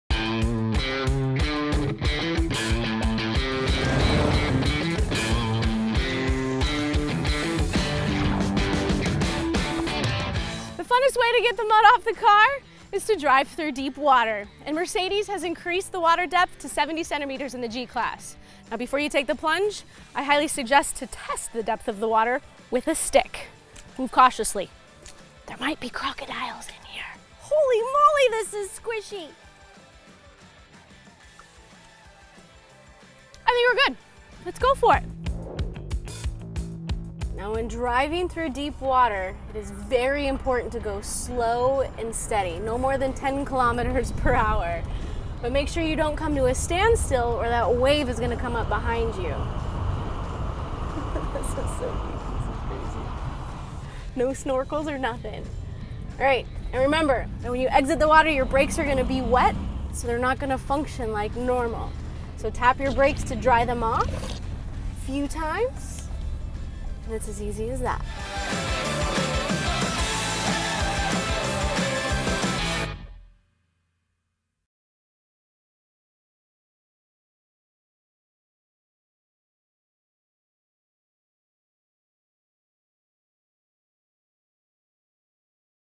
El Mercedes Clase G nos enseña a vadear correctamente | CAR AND DRIVER
El todoterreno más emblemático de la firma alemana se ha puesto el mono de trabajo y nos ha enseñado en tres sencillos pasos la mejor forma de vadear terrenos difíciles con total naturalidad y sin ningún tipo de problema.